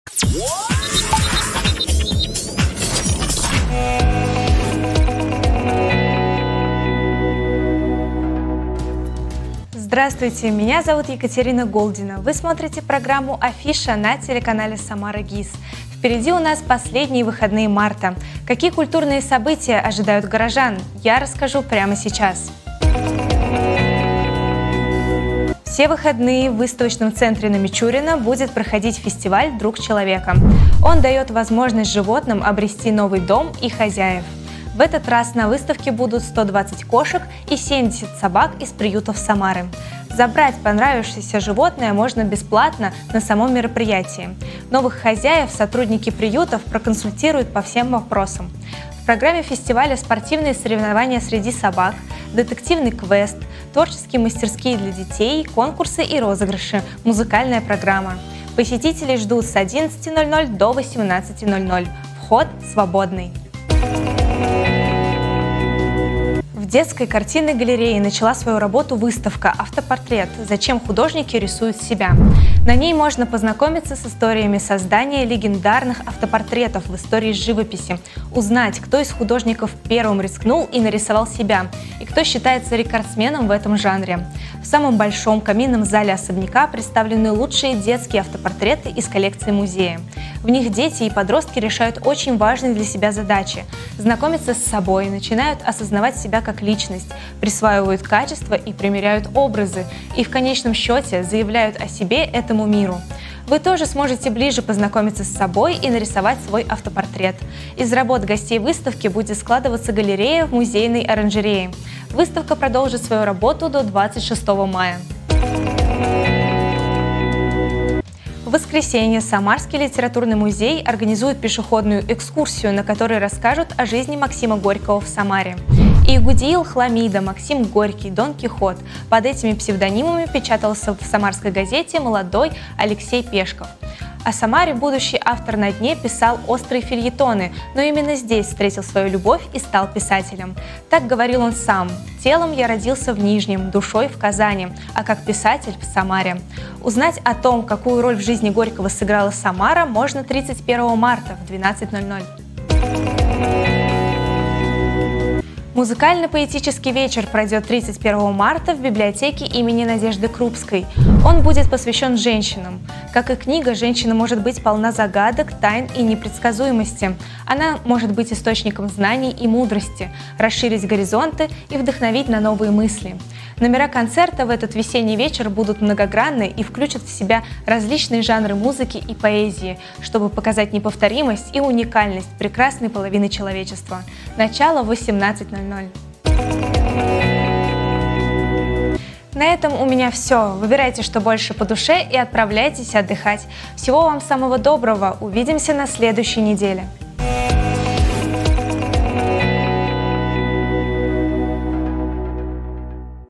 Фестиваль "Друг человека", пешеходная экскурсия / "Афиша" от 29.03.2024
Афиша: куда сходить в Самаре 30 и 31 марта.